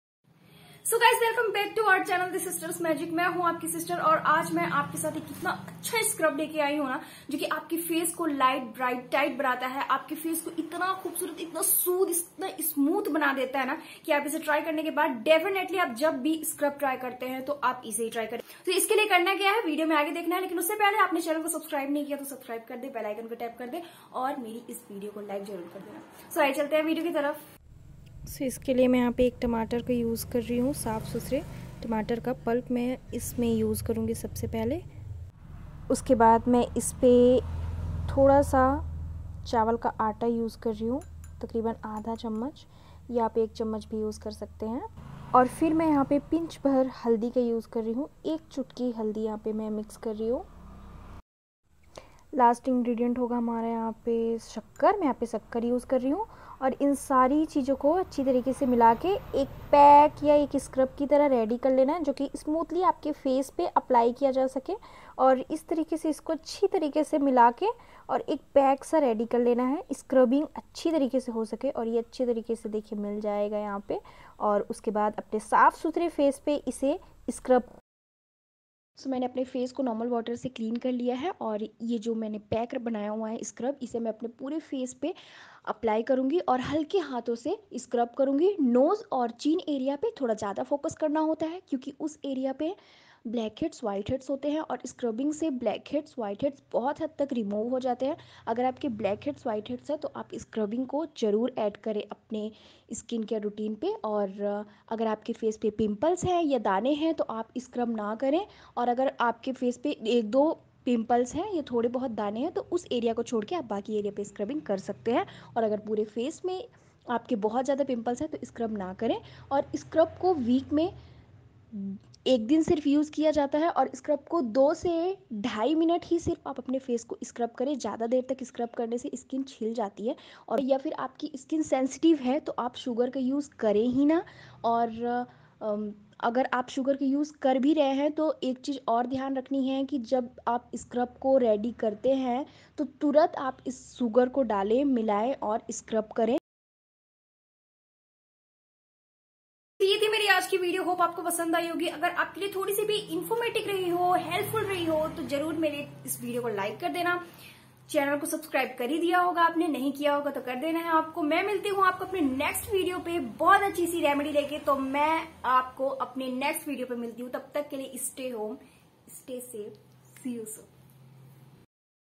Rice Scrub, Glowing Tomato Facial Scrub, 2 in 1 Remedy for glow and Scrub, get Fair skin instantly
1 tomato juice, 1 teaspoon rice flour, 1 pinch turmeric, 1 teaspoon sugar, after mixing all these ingredients, clean your face and apply it well and massage it with light hands, then wash the face thoroughly.  With this your face will be cleaned well, there will be a glow in your face and black heads and white heads will come out